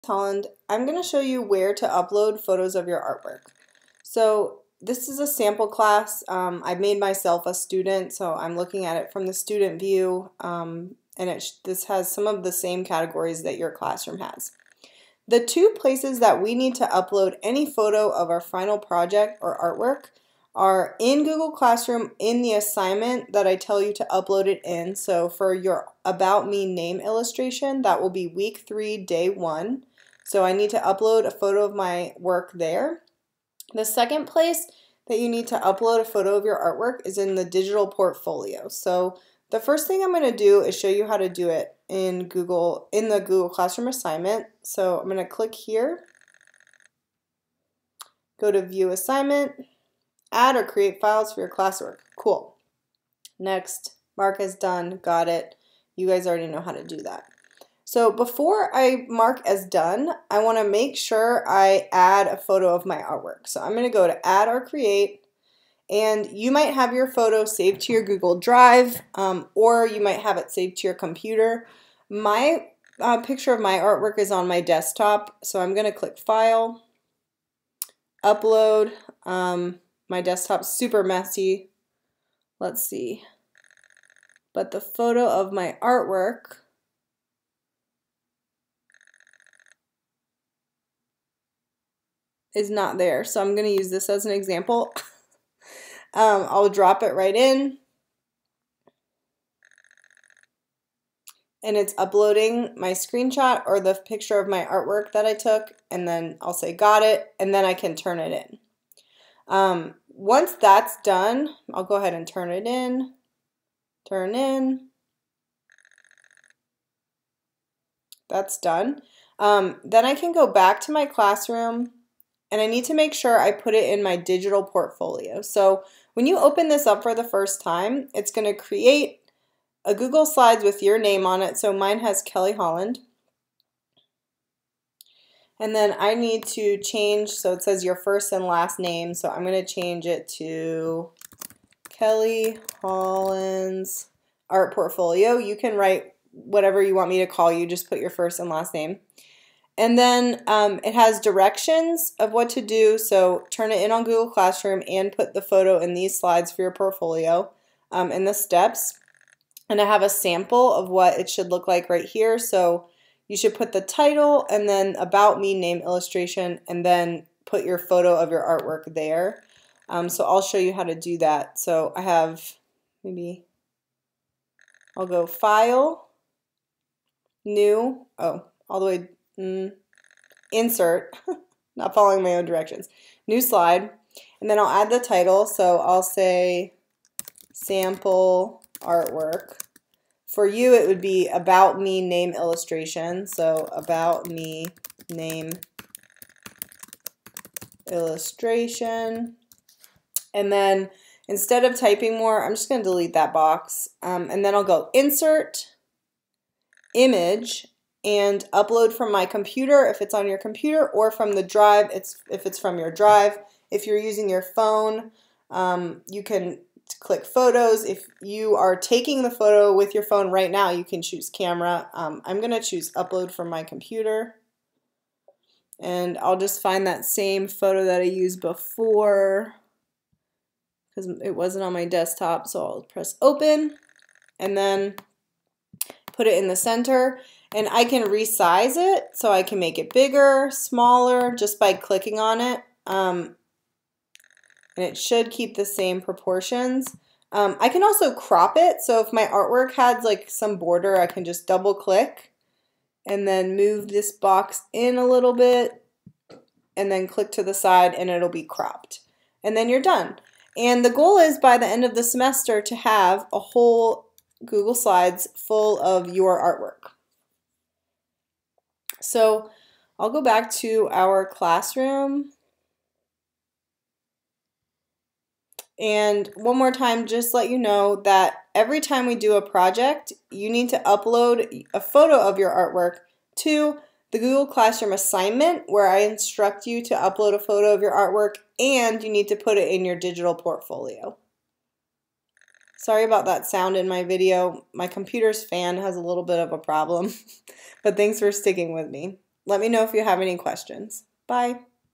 Where to Upload Photos of Artwork
There are two places you need to upload photos of your artwork:
- In the Google Classroom Assignment
- In your Digital Portfolio.